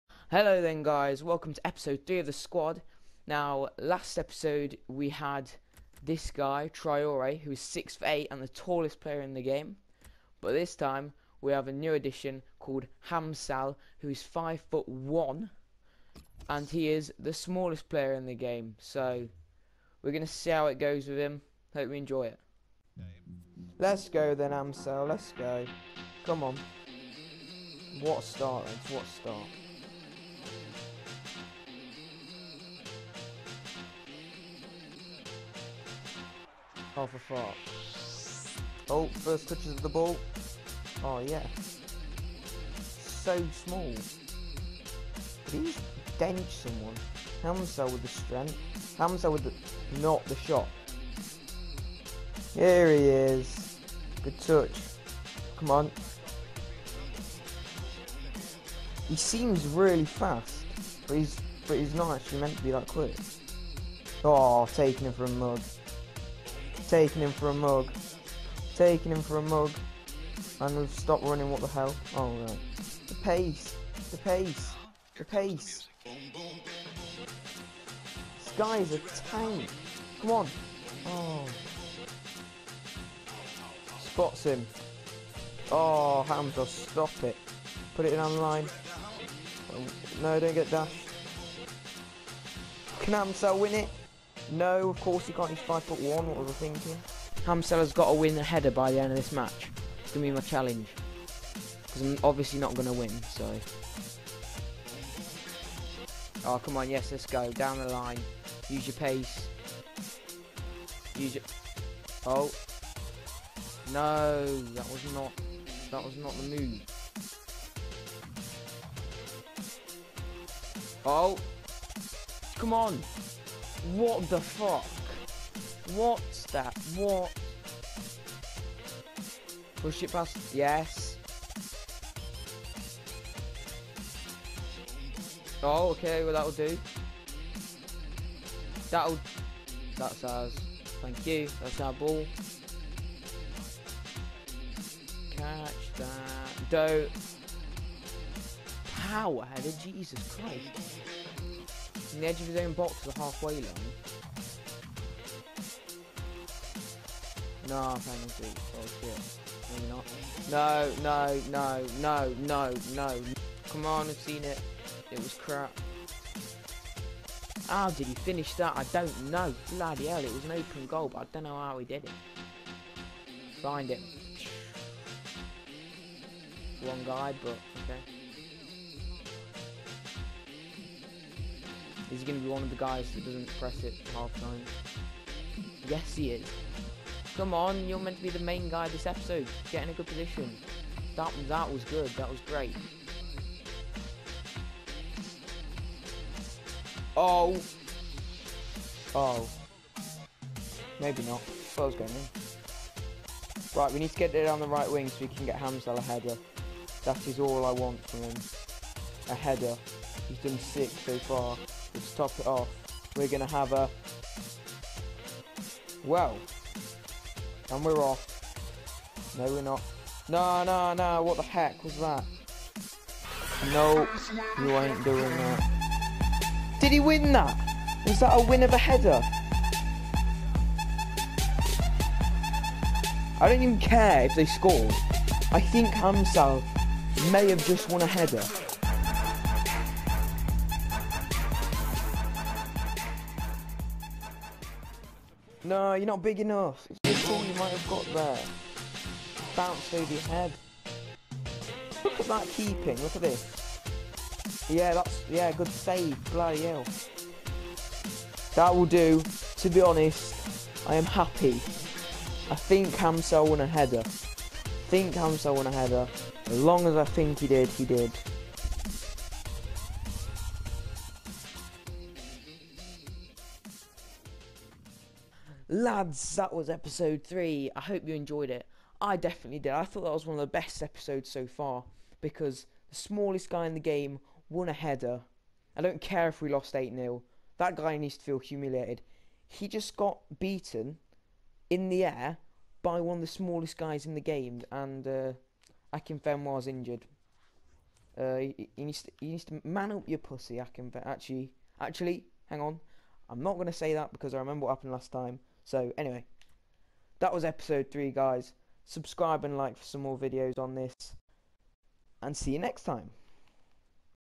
The SQUAD Ep3- Hamsal "He wins a header!"
Hello people and welcome to the 3rd episode of the squad we have now got our front three players in our squad and we are getting ever closer to entering the cup with this interesting squad, if you guys are enjoying this series it would be amazing for you to show this through LIKING or SUBSCRIBING or even leaving a nice COMMENT